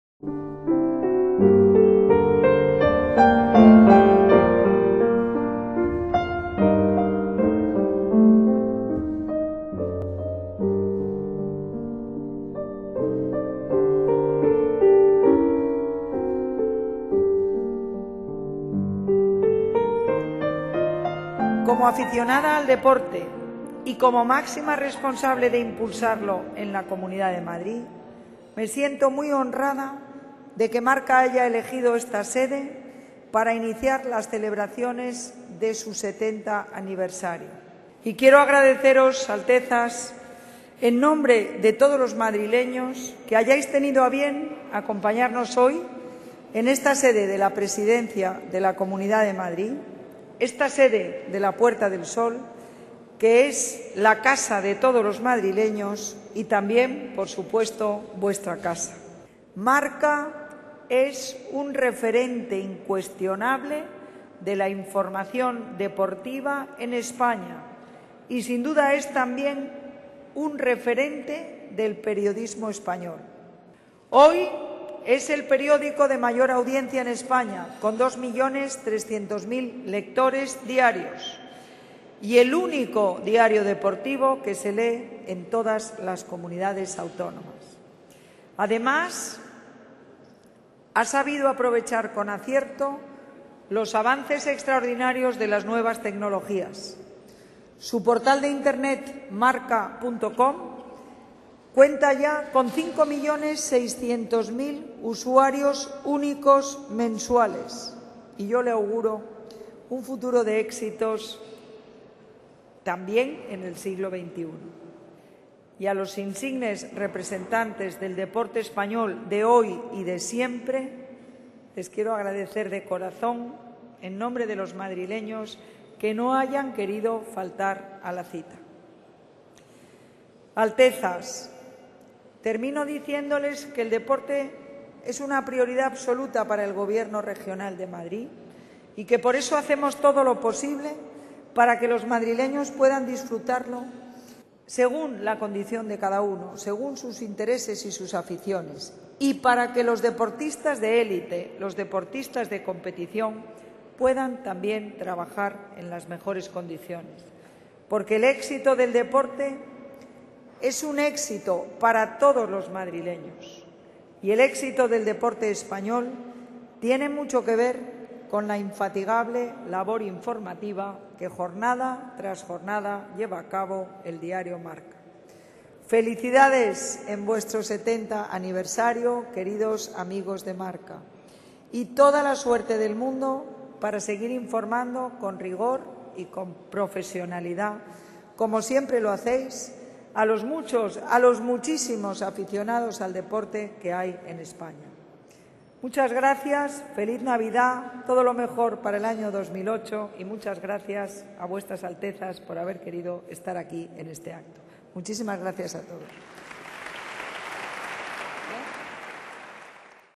La Presidenta asiste celebración 70 aniversario "Marca"
La Presidenta de la Comunidad de Madrid, Esperanza Aguirre, asiste a la celebración del aniversario del diario deportivo "Marca", que conmemora el próximo año sus setenta años de existencia. Aguirre es acompañada por el Consejero de Deportes, Alberto López Viejo.